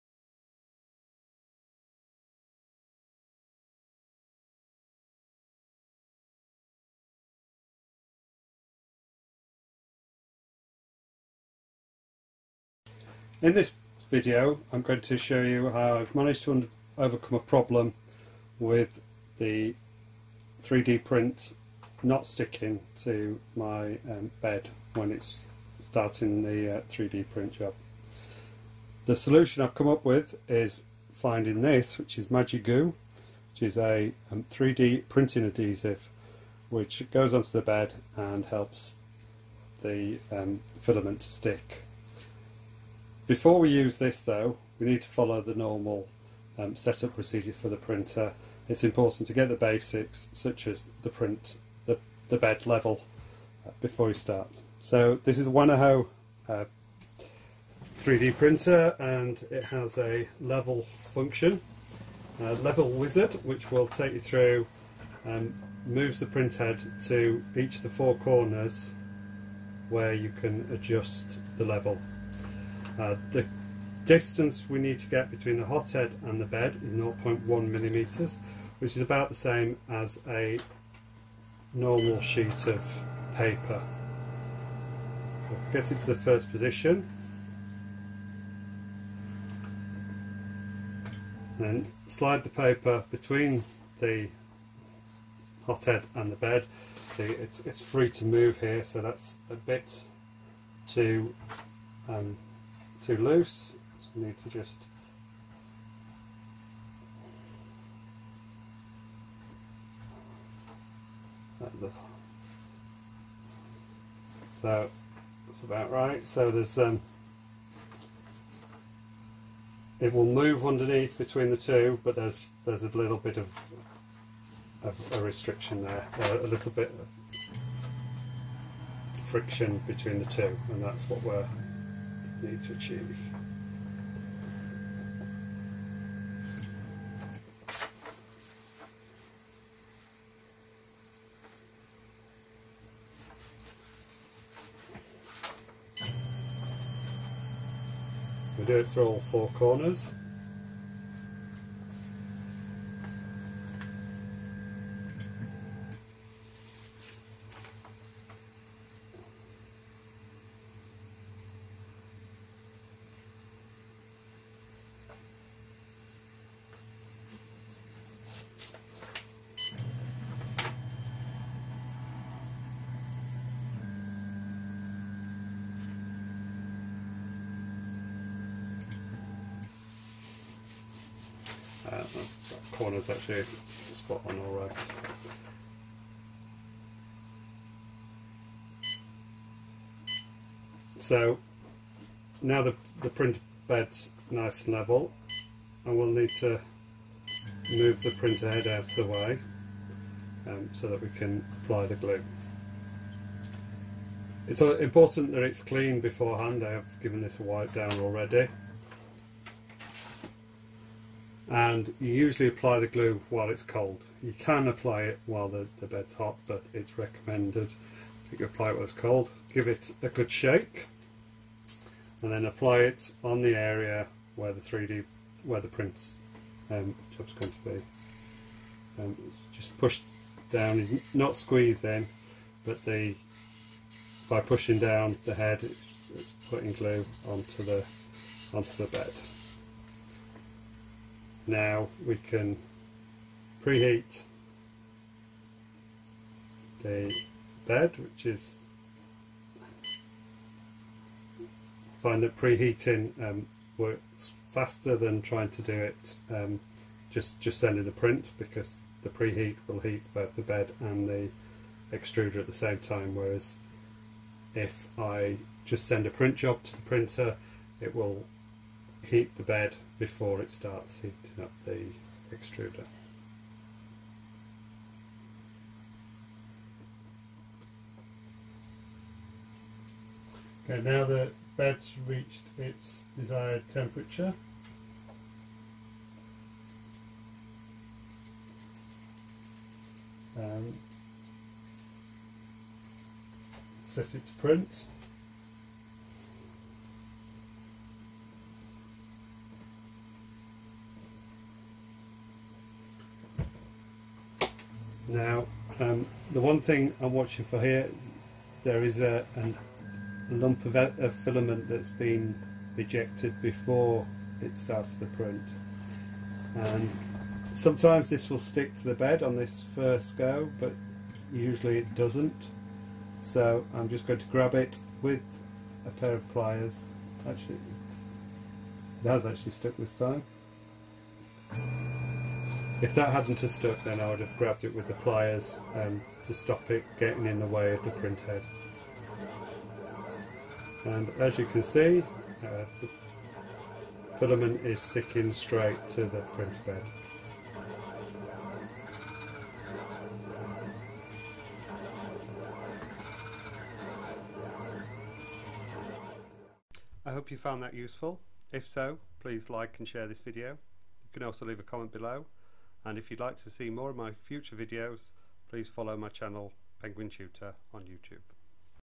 3D Printer Glue - fix first layer 3D print problems
Having had lots and lots of failed prints on my 3D printer I've been in search of a soluttion the first layer printing problems. I've discovered a glue that actually works for getting the 3D print to work first time. It helps the first layer of the filament stick to the bed and can be easily removed using a damp cloth afterwards.